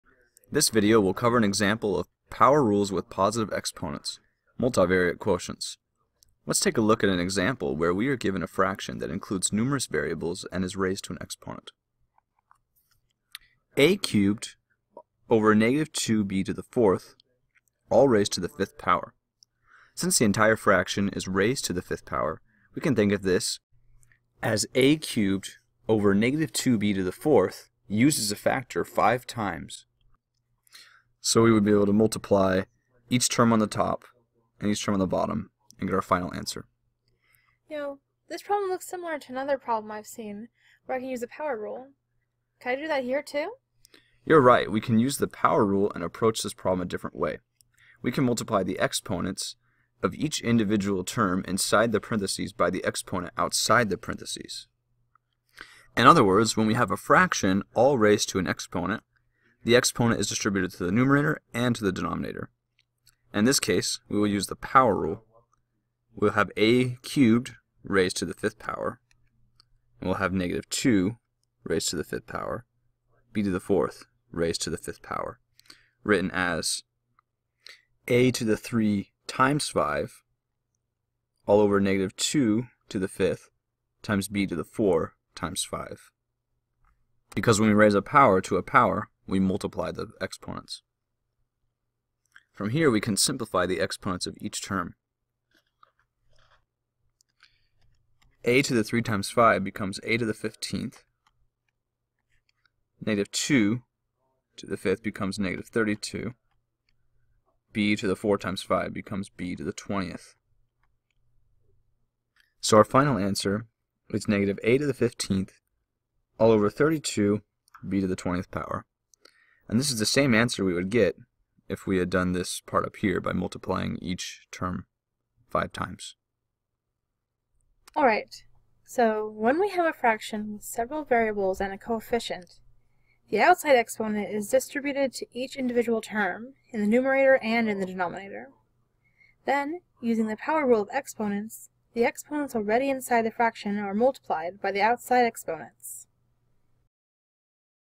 Topic: Power Rules with Positive Exponents: Multivariate Quotients
This is a topic level video of Power Rules with Positive Exponents: Multivariate Quotients for the ASU College Algebra and Problem Solving Course.

Join us!